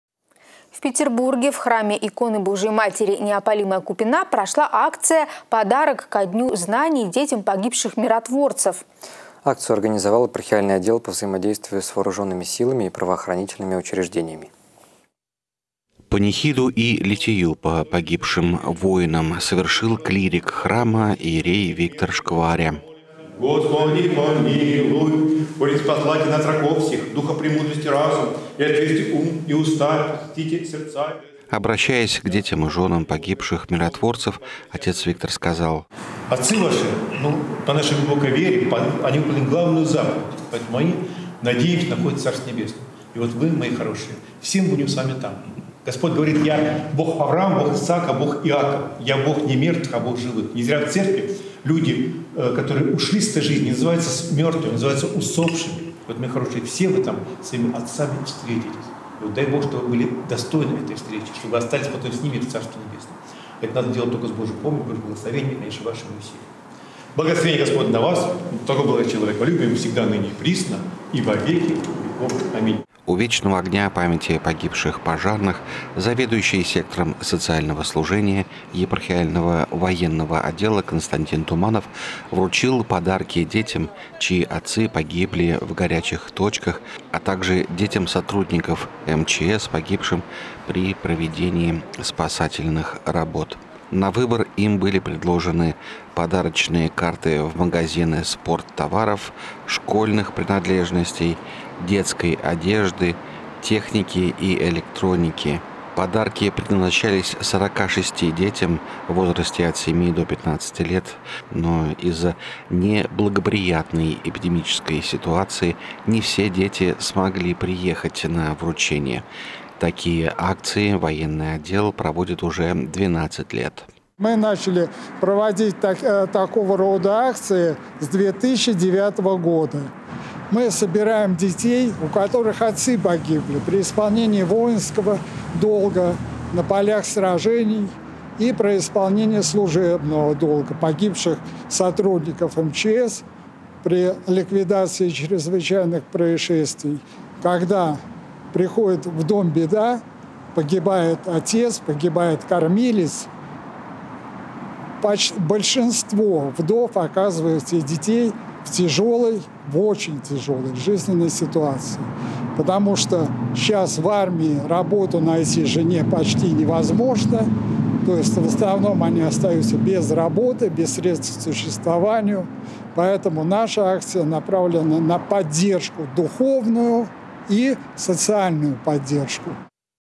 В Петербурге прошла акция «Подарок ко Дню знаний детям погибших миротворцев».
В Петербурге в храме иконы Божией Матери «Неопалимая купина» прошла акция «Подарок ко Дню знаний детям погибших миротворцев». Акцию организовал епархиальный отдел по взаимодействию с Вооруженными Силами и правоохранительными учреждениями.